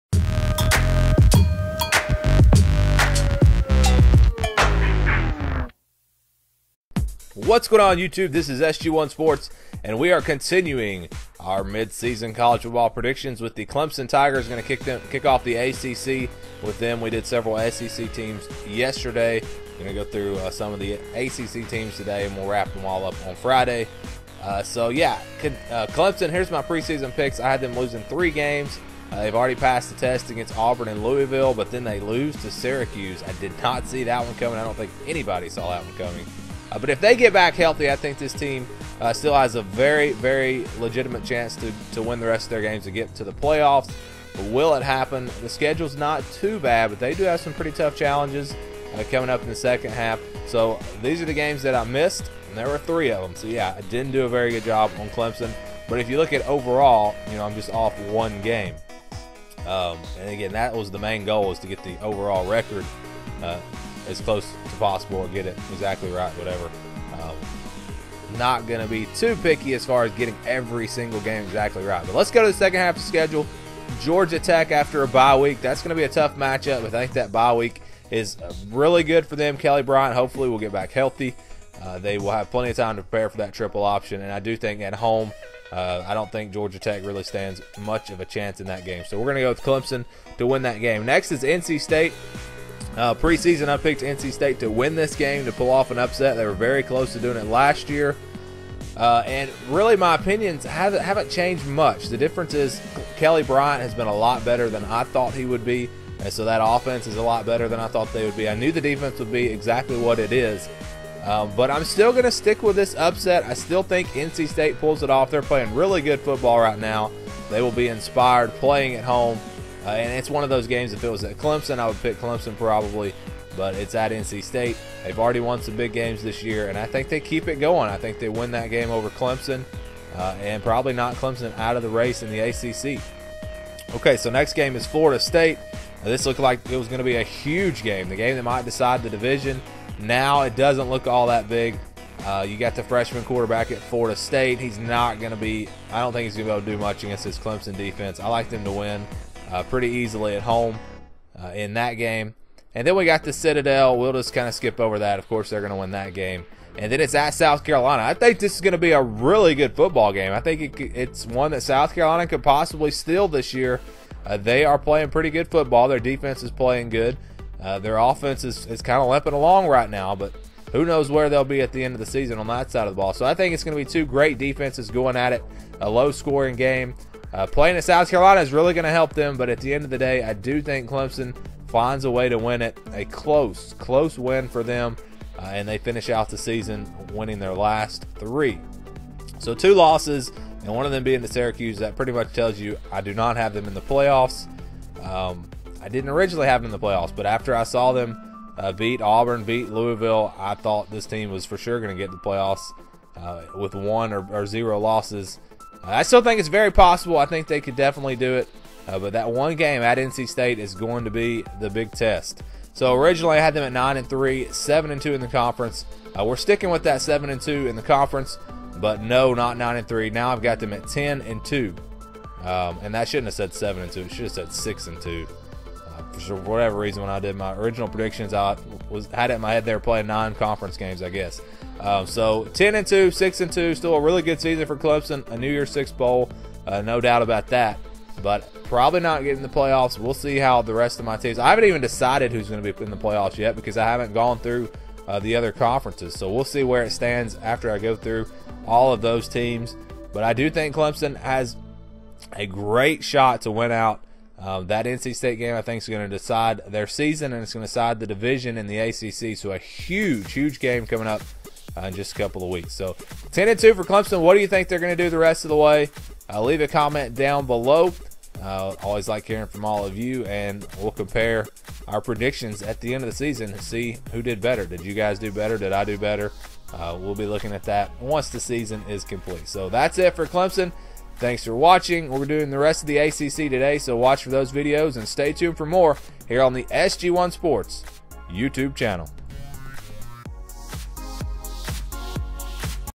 Clemson Football - Mid-Season Predictions
In this video, we look back at how our preseason predictions are doing so far for Clemson and use this opportunity to make changes to our predictions for the second half of the season.
Sports Gaming 1 is a channel that combines the real life world of sports with the video game world.